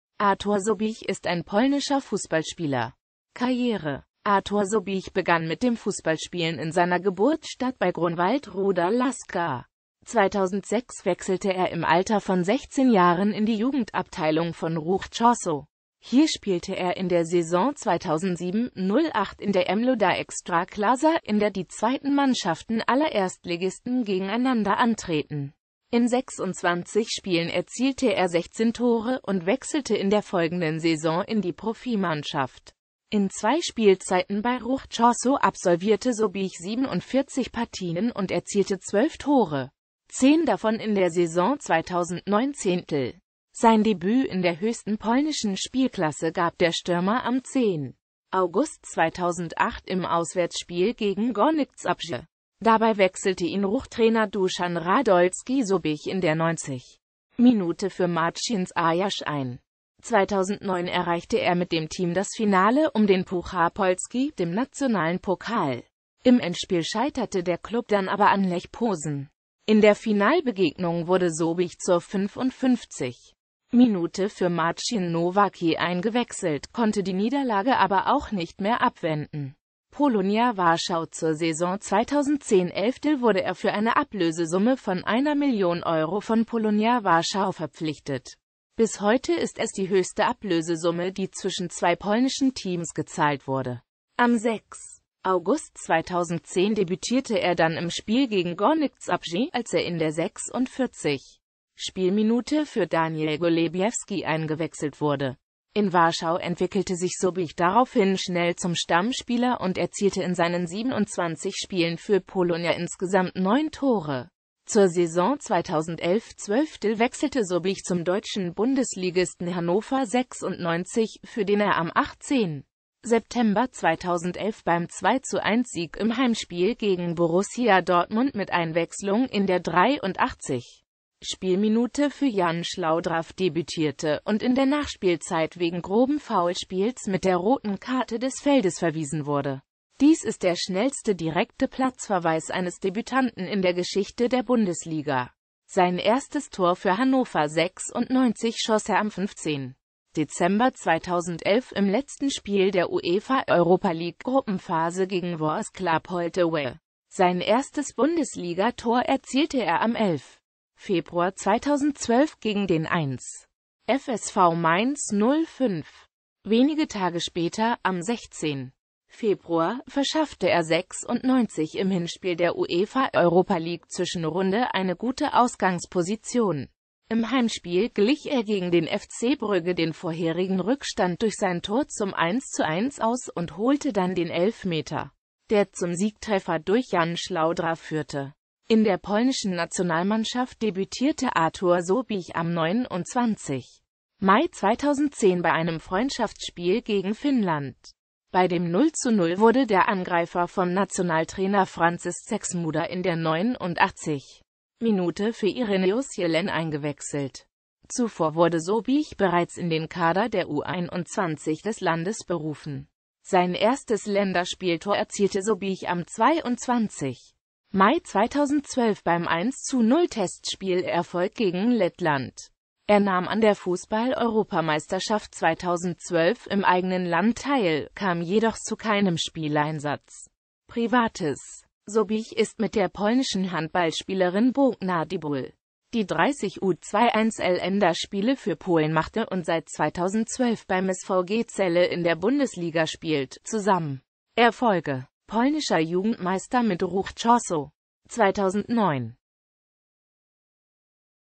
Artur Sobiech
Vielen Dank für Ihre Unterstützung:

Artur Sobiech (* 12.Juni 1990 in Ruda Śląska) ist ein polnischer Fußballspieler.